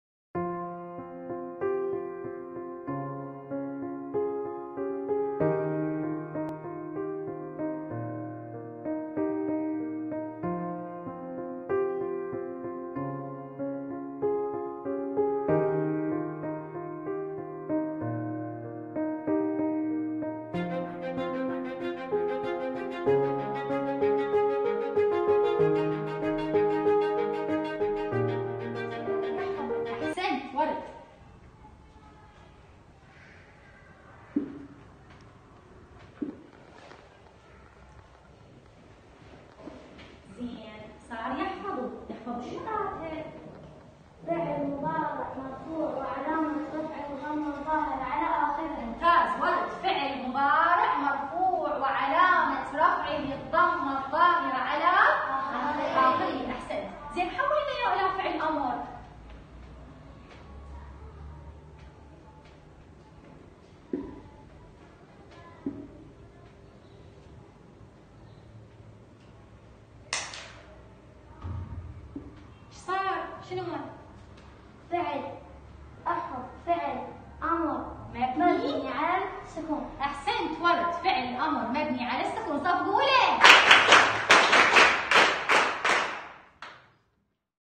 درس اللغة العربية بأشراف ست سجى المدرسة العراقية الالمانية _ ذي قار
Er lernte Arabisch unter der Aufsicht von sechs Saja # Iraqi-German School _ Dhi Qar

He studied Arabic under the supervision of six Saja # Iraqi-German School _ Dhi Qar